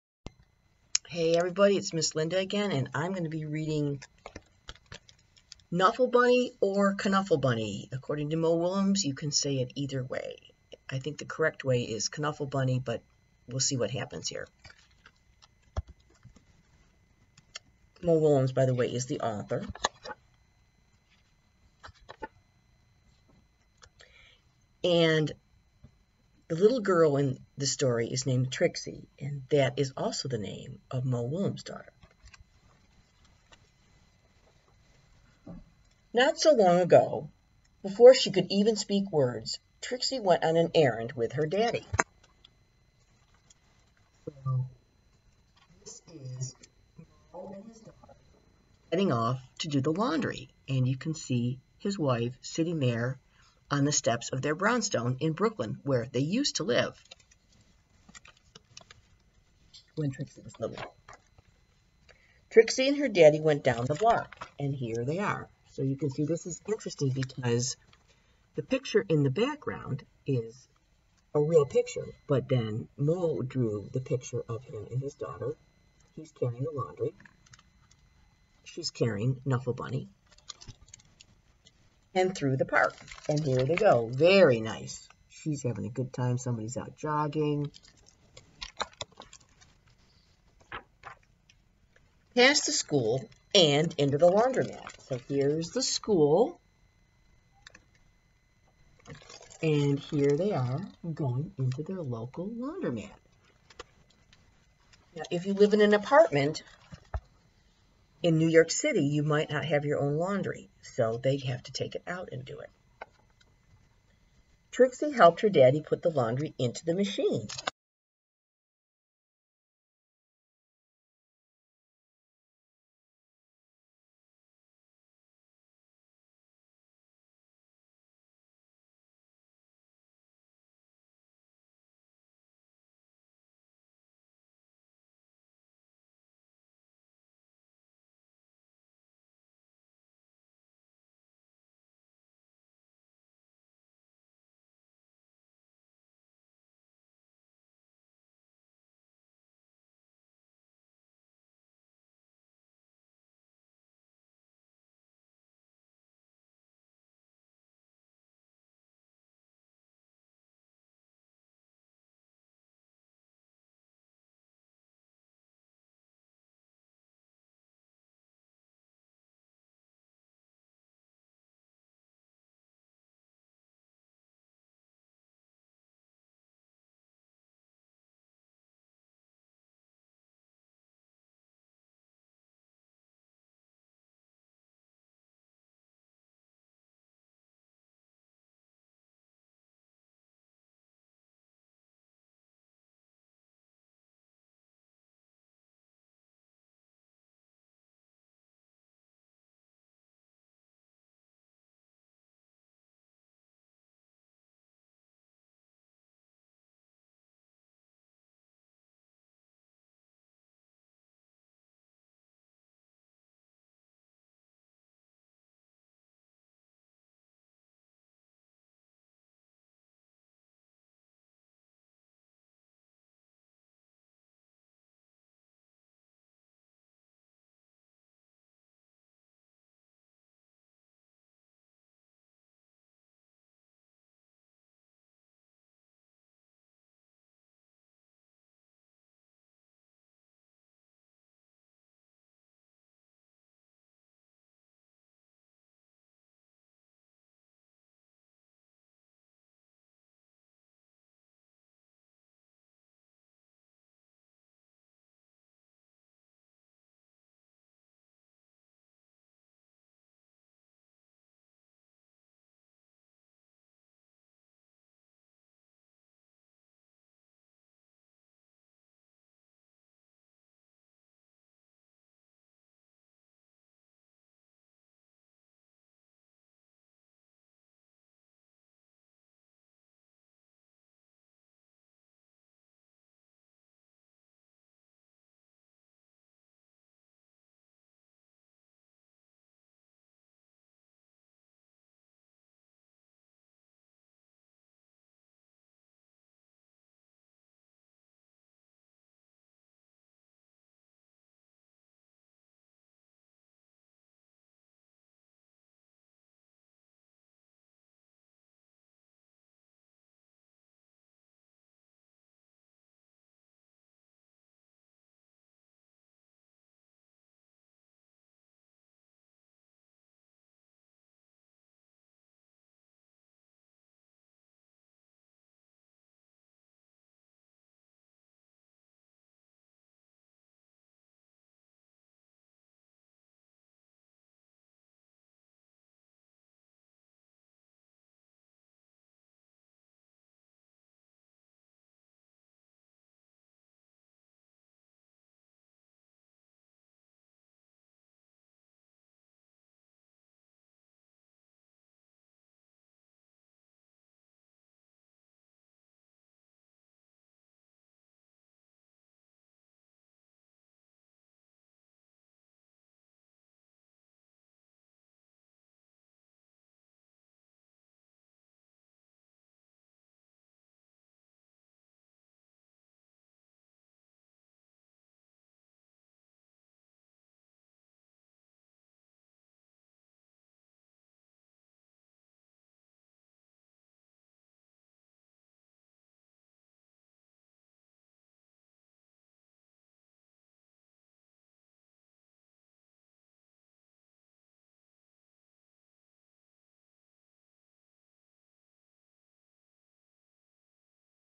Knuffle Bunny
Miss Linda read KNUFFLE BUNNY by Mo Willems